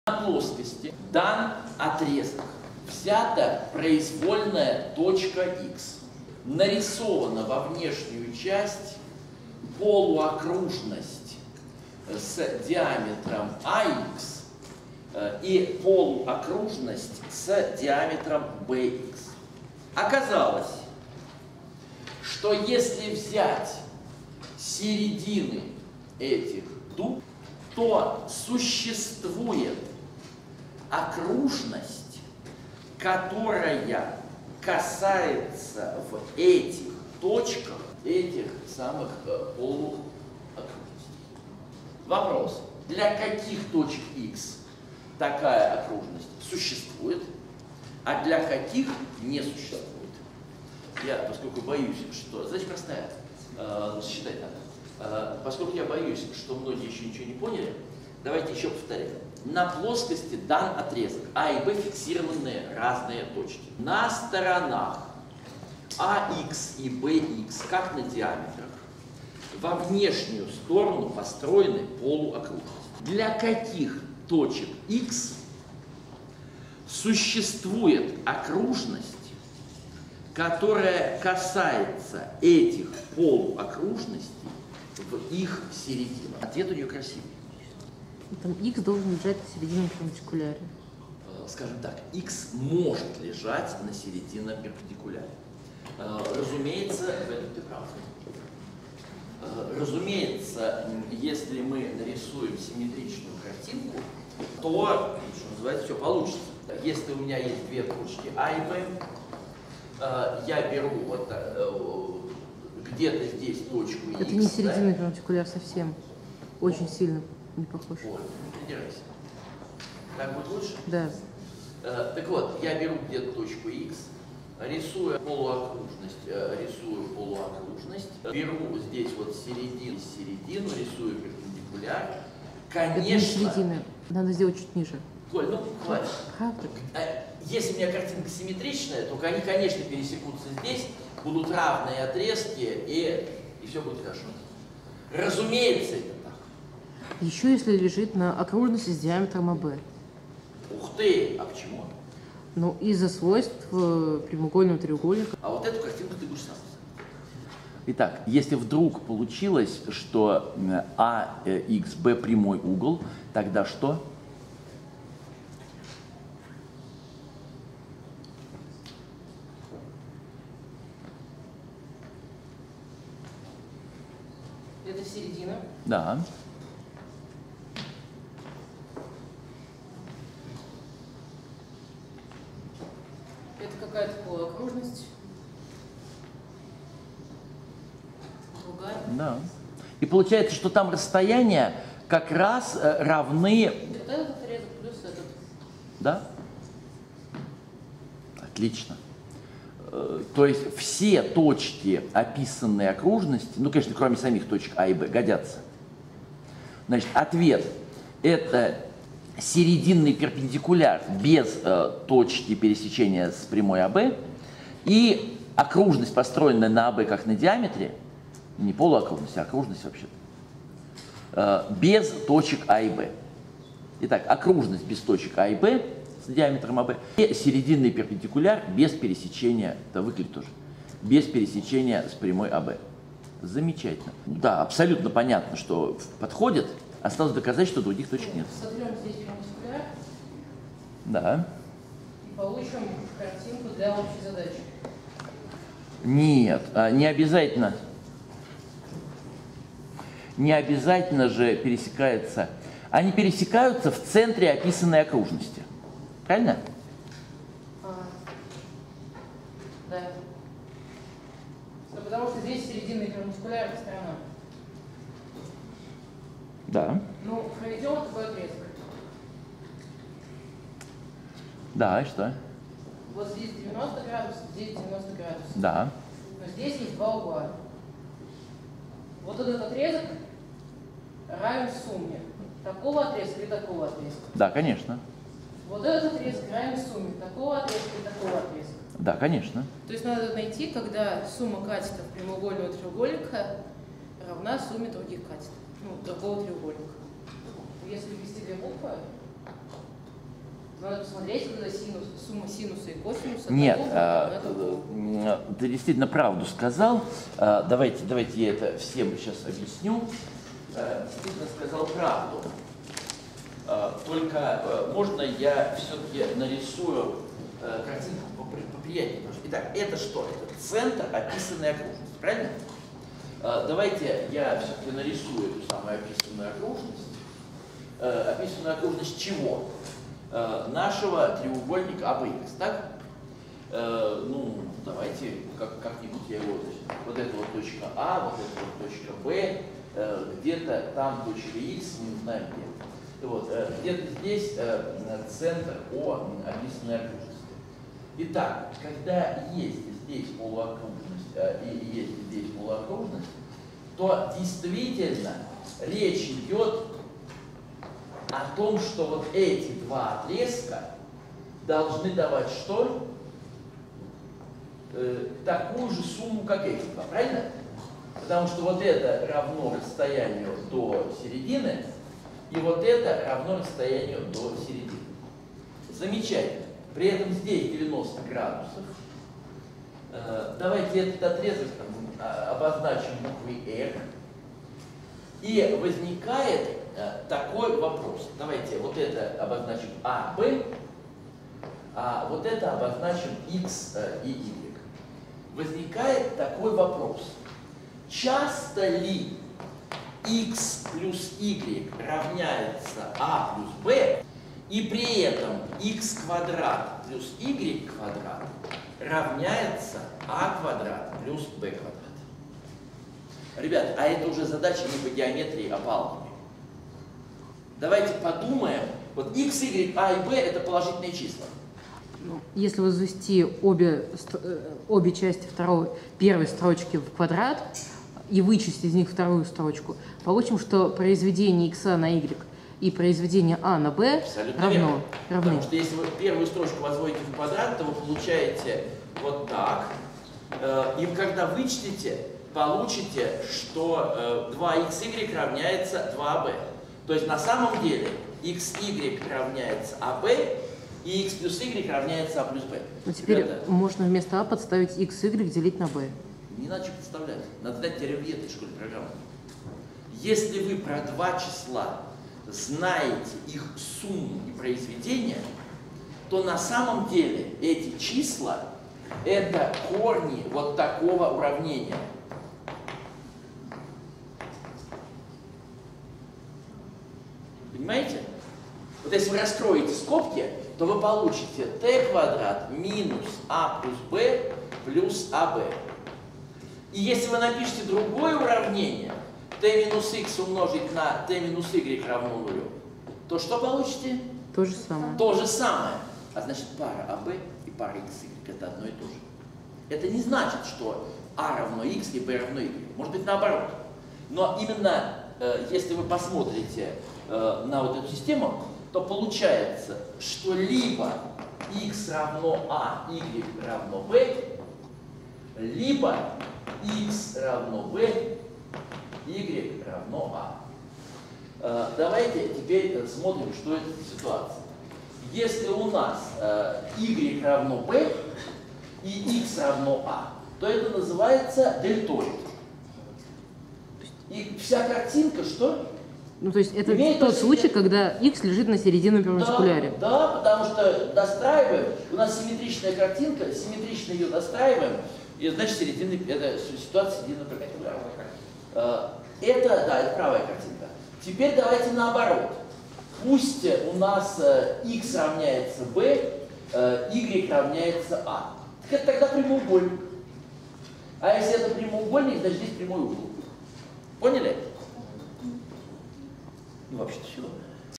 М1903. Окружность, касающаяся полуокружностей в их серединах
На плоскости дан отрезок AB. На сторонах AX и BX треугольника ABX как на диаметрах внешним образом построены полуокружности. Найдите множество таких точек X, что существует окружность, касающаяся этих полуокружностей в их серединах.